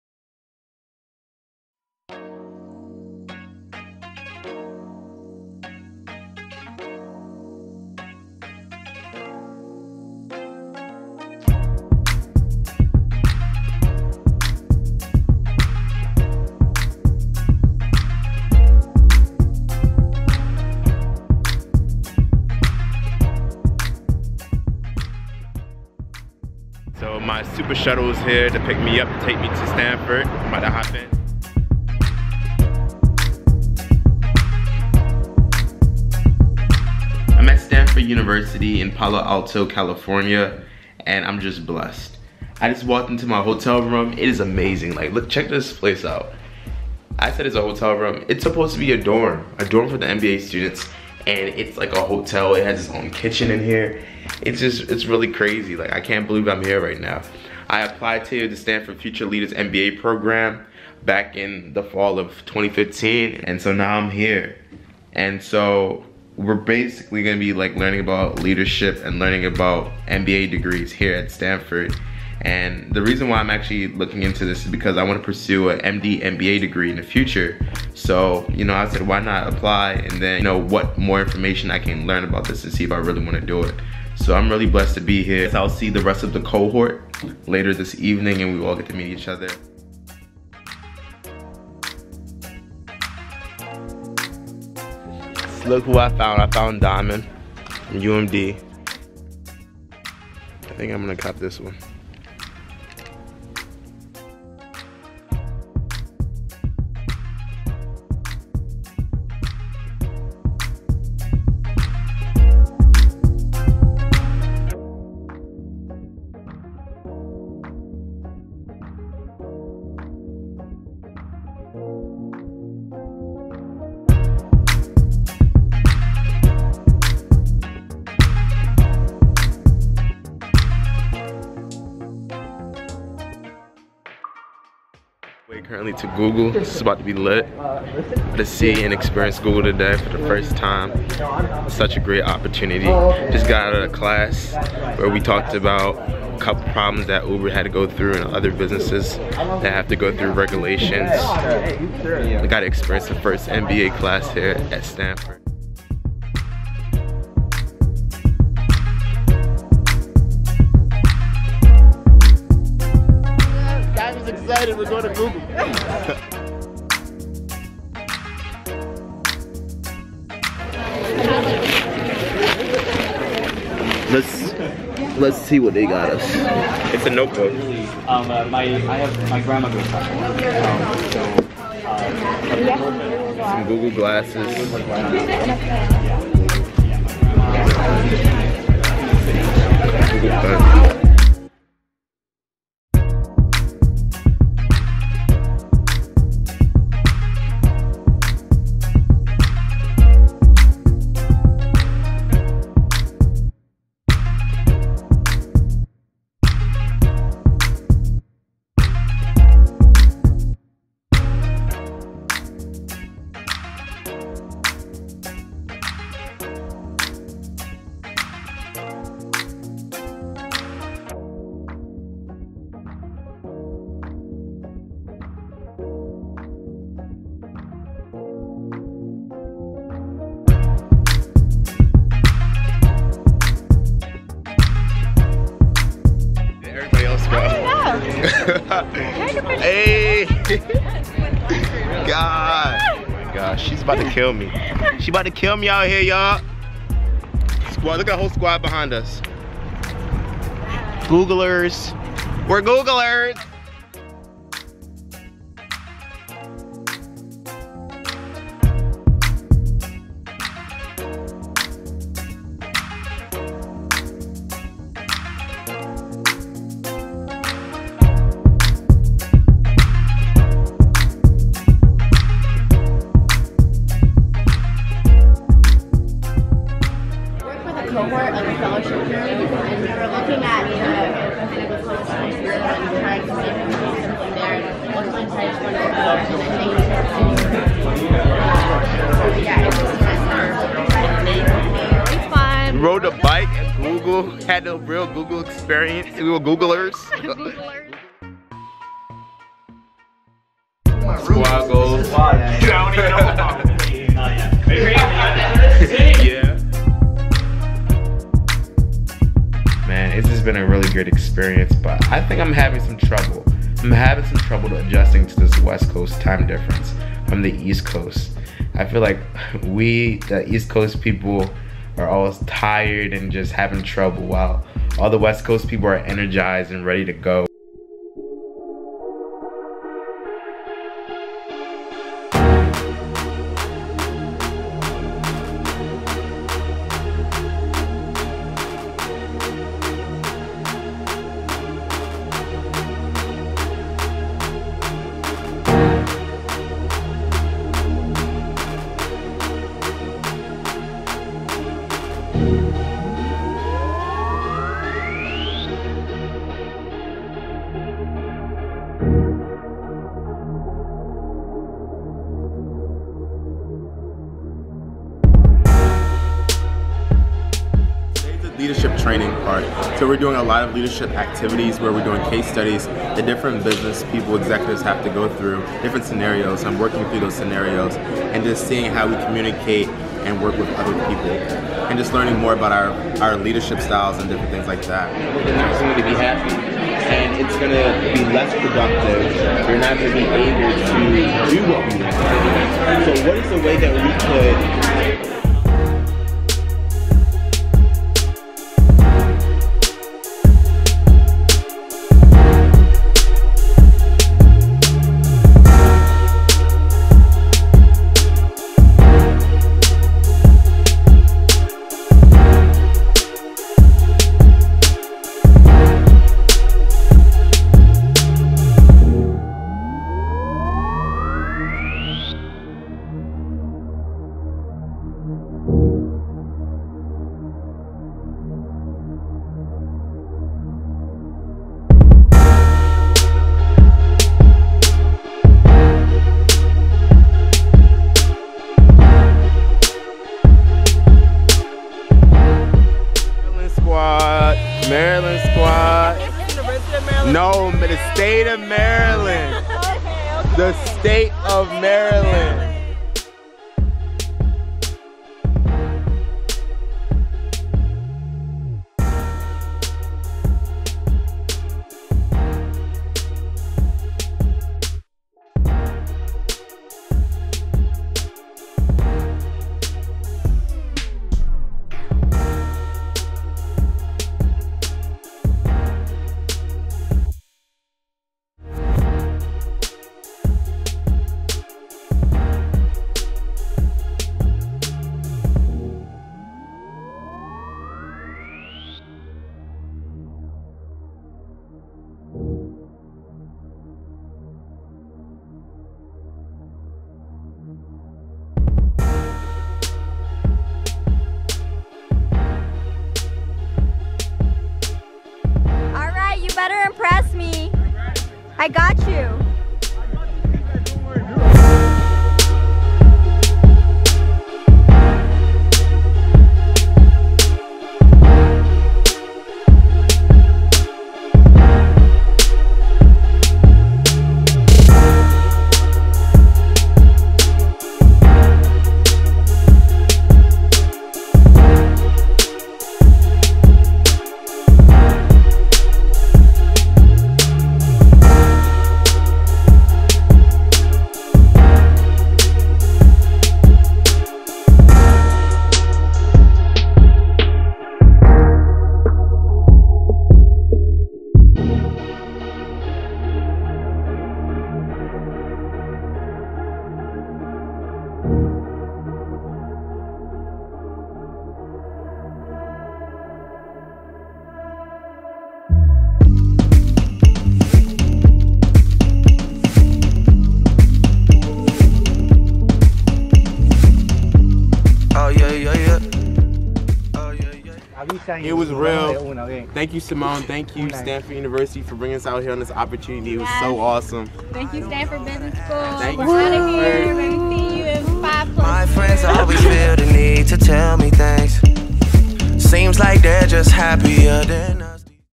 My Trip to Stanford University: Stanford Future Leaders Program!
Watch this video as I show my experience at Stanford University this summer. I participated in the Stanford Future Leaders MBA Program... MBA? Yes, I am currently interested in pursuing an MD/MBA degree so I sought out this opportunity to explore getting a MBA degree at Stanford University.